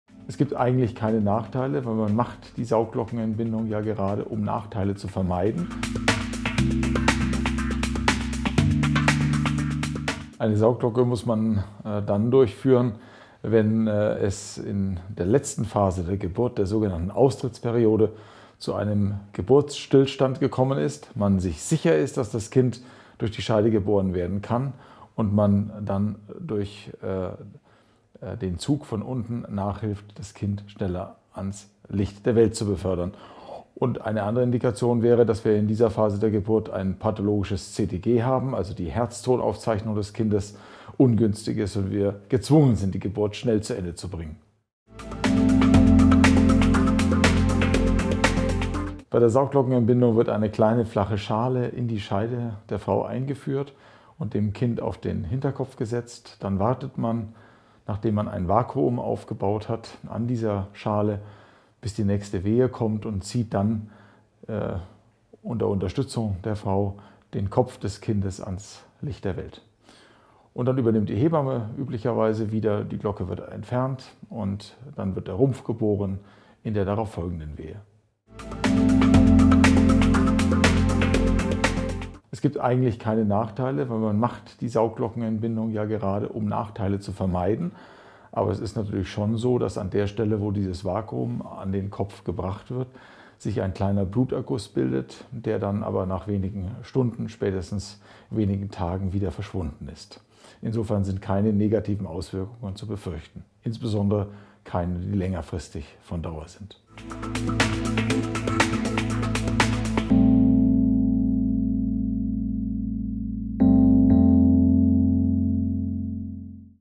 Saugglocke: 3 Fragen 3 Antworten | Asklepios
Die Saugglocke kommt bei der Geburt zum Einsatz, wenn das Baby beim Austritt aus dem Geburtskanal Unterstützung von außen benötigt. Eine Saugglockengeburt ist nur bei einem Kind in der normalen Kopflage möglich. Prof. Dr. Holger Maul, Chefarzt für Frauenheilkunde, Geburtshilfe und Pränatalmedizin an den Asklepios Kliniken Barmbek und Wandsbek
erklärt, wann die Saugglocke eingesetzt wird, wie eine Saugglockenentbindung verläuft und ob es Nachteile oder Nebenwirkungen beim Einsatz einer Saugglocke gibt.